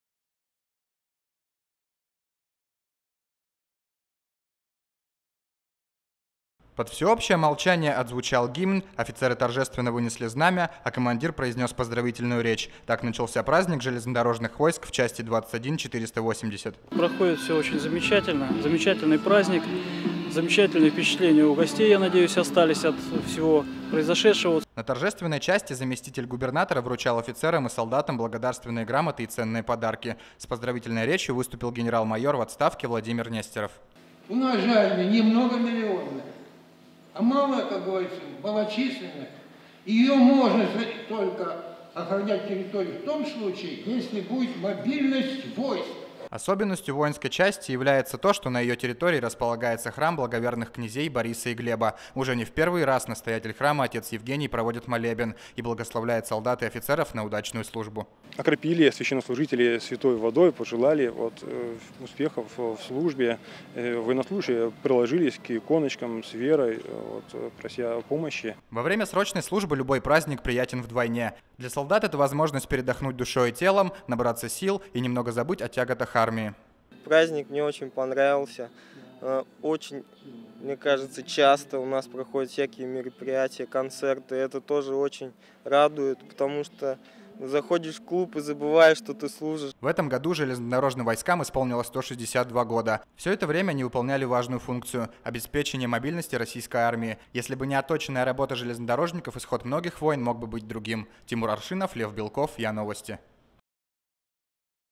В Ярославле отметили День ЖДВ
В этом году железнодорожным войскам исполнилось 160 лет.